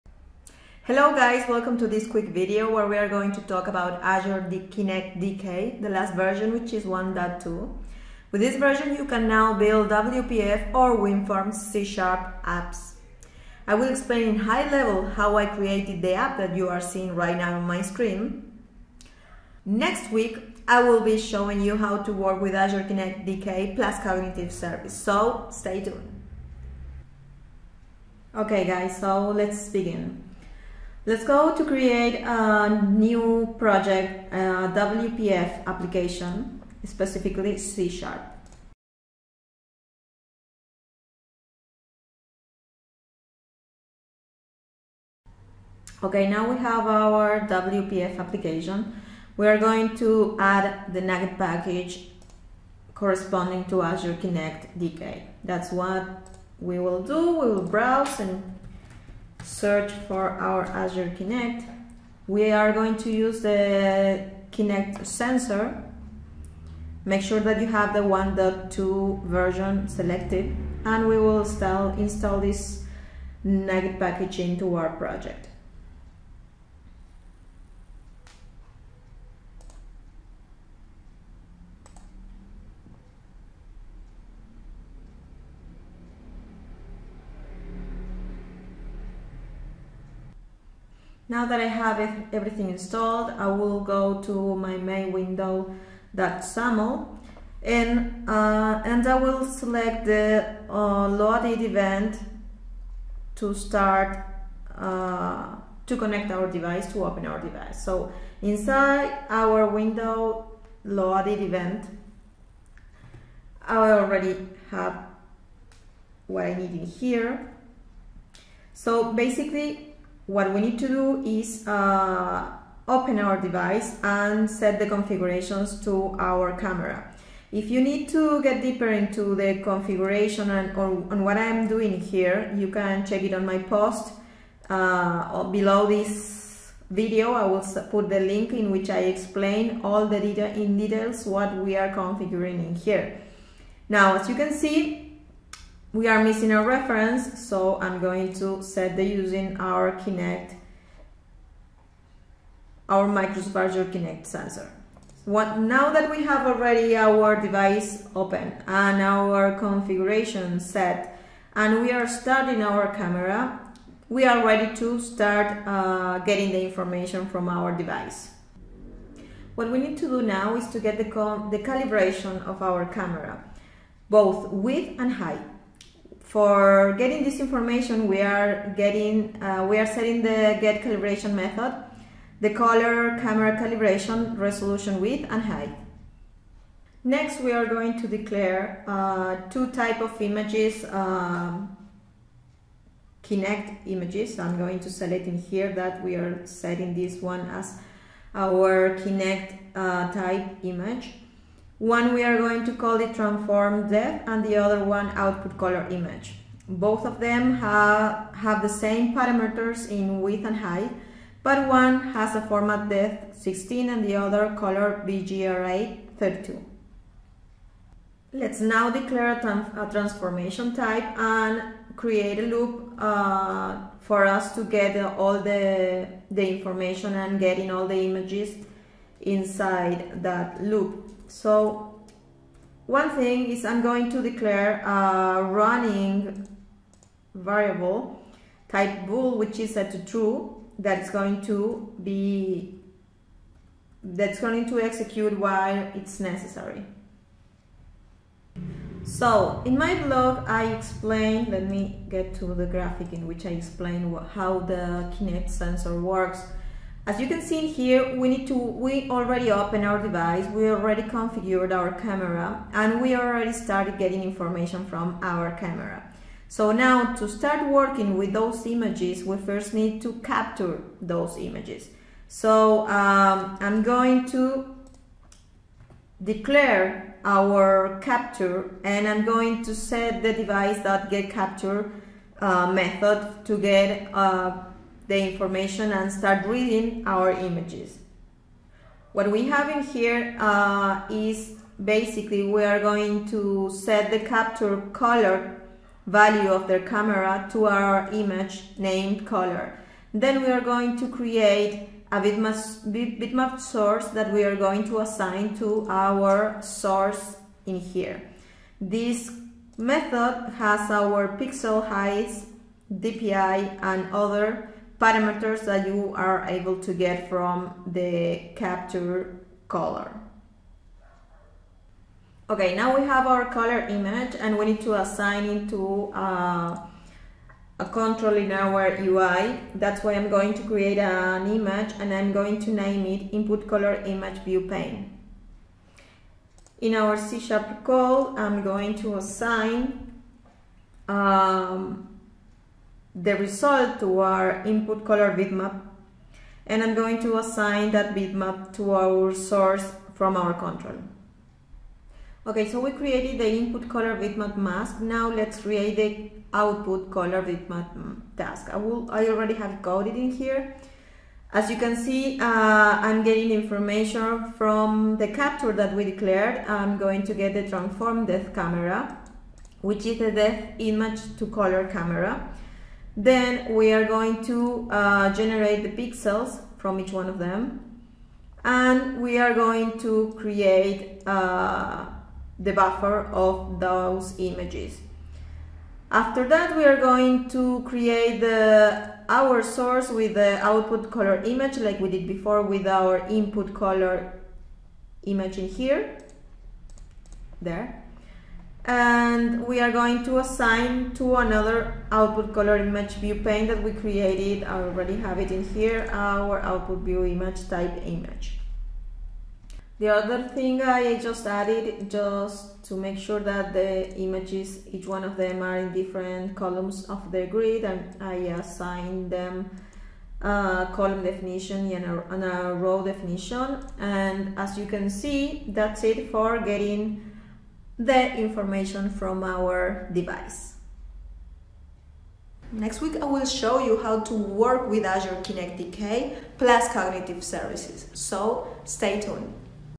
First Azure Kinect App–VIDEO- Getting Depth and Color camera
Lets create a WPF app... to display our Depth and Color camera from our Azure Kinect Device